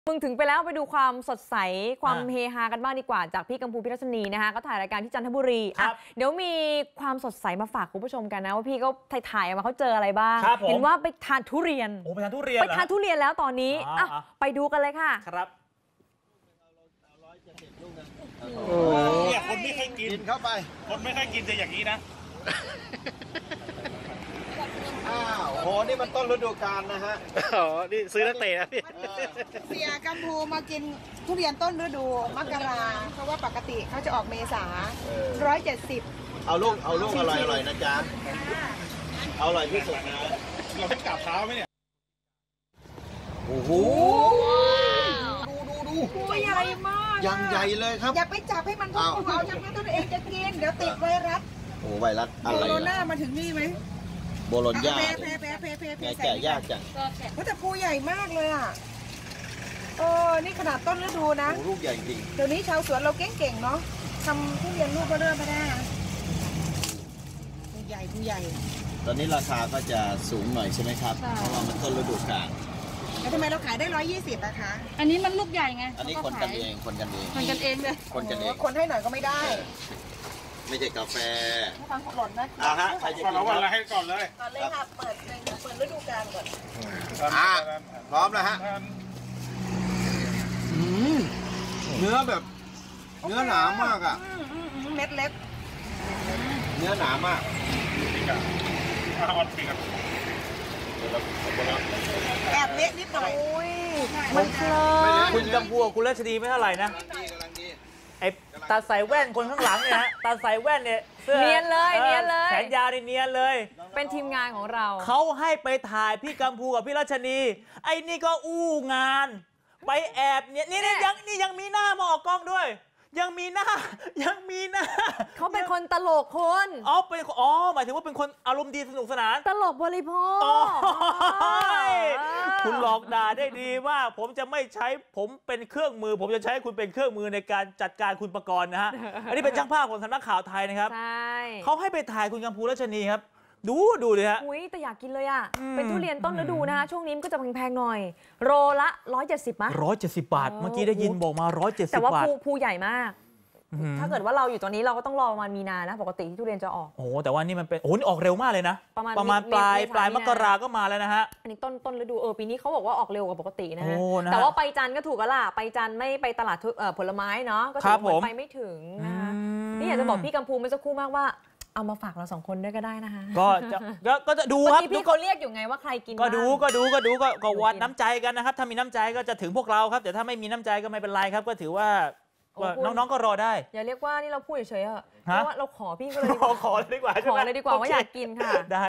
"กำภู – รัชนีย์" แวะตลาดผลไม้ “ทุเรียนจันท์” เริ่มออกขายแล้ว
พิธีกรคู่ขวัญ "กำภู - รัชนีย์" ไปถ่ายรายการที่ จ.จันทบุรี แวะตลาดผลไม้ เพราะทุเรียนเริ่มออกแล้ว ปีนี้แม่ค้าบอกว่าออกเร็วกว่าทุกปี แต่ราคาก็ยังสูงอยู่ 170 บาท/ กก. คลิก